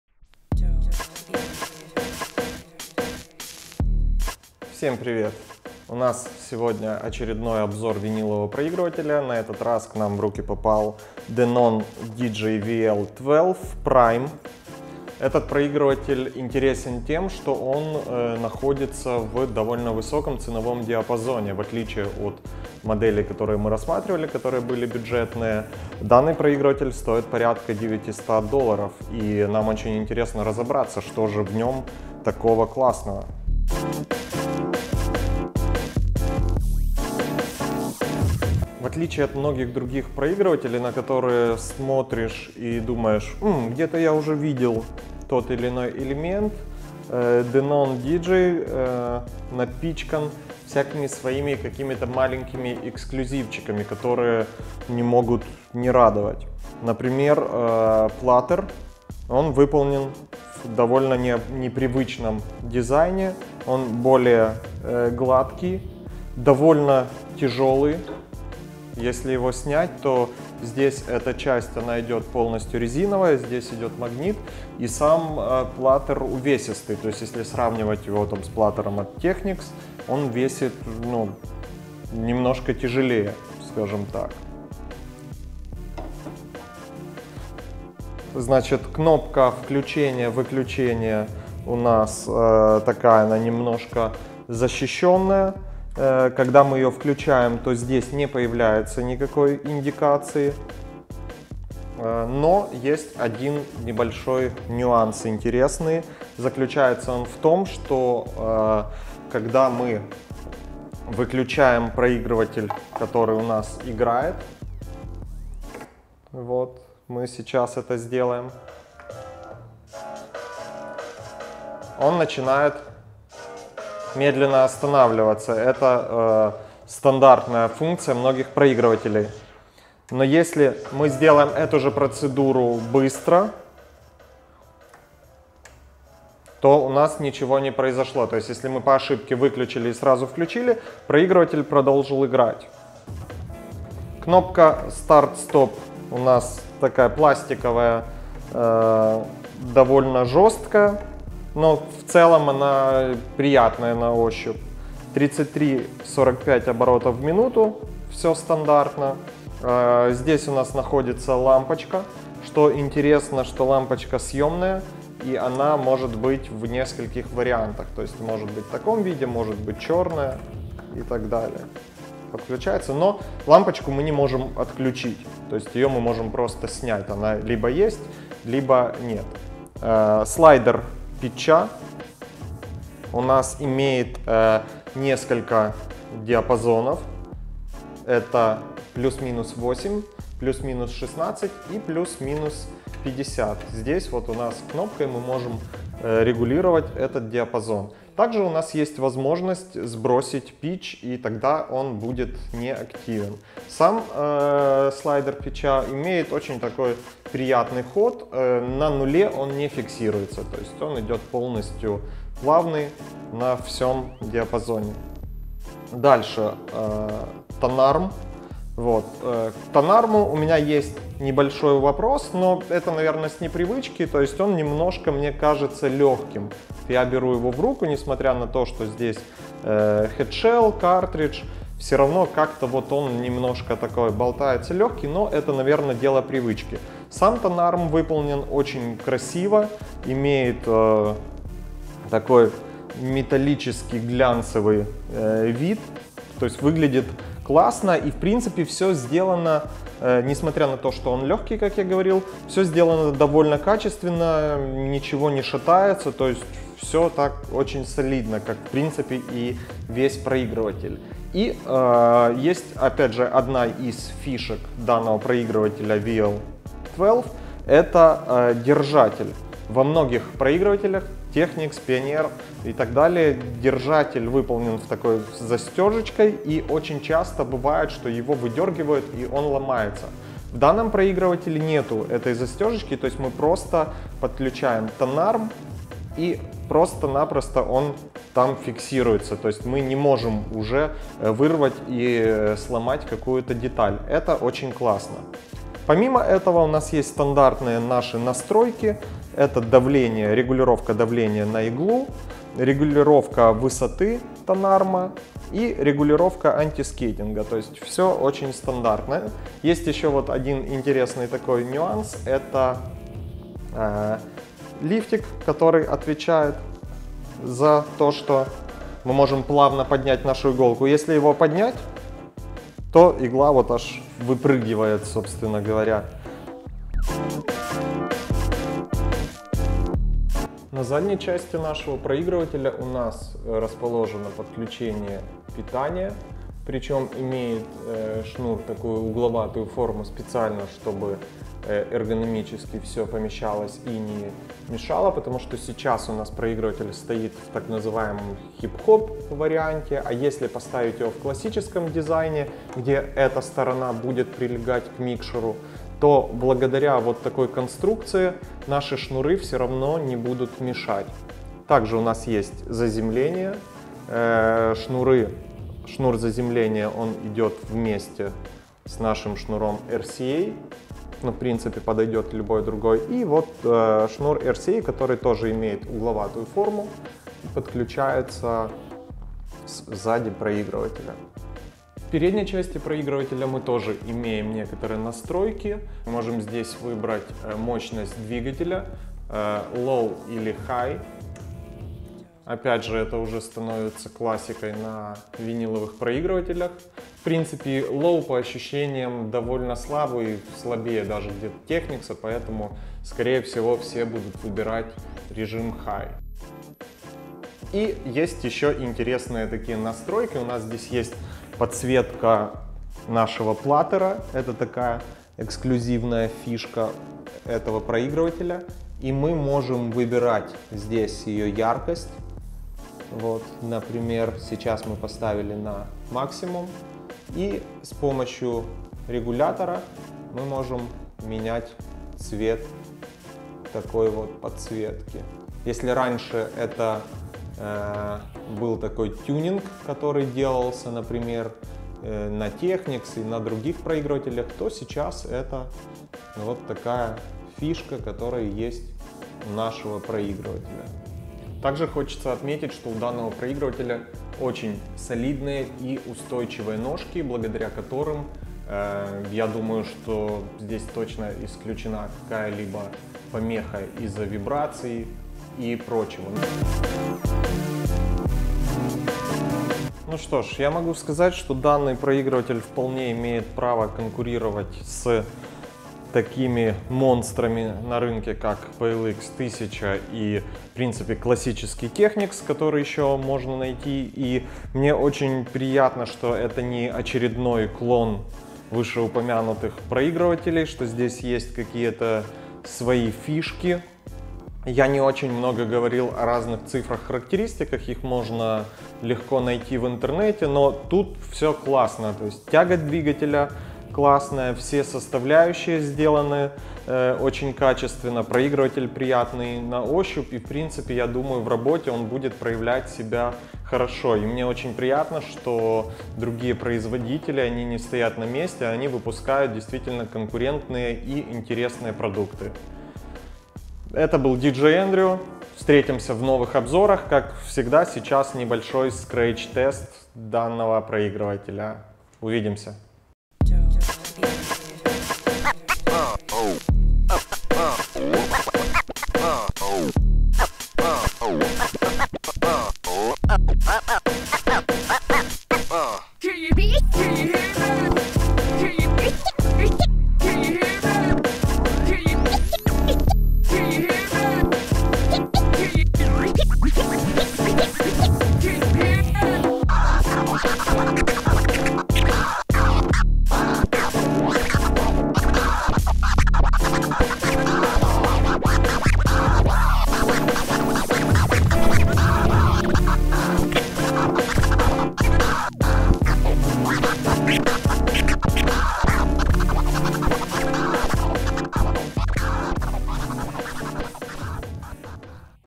Обзор винилового проигрывателя Denon VL12 Prime Turntable Review
Очень интересный виниловый проигрыватель, который не похож на других конкурентов и со своими особенными функциями.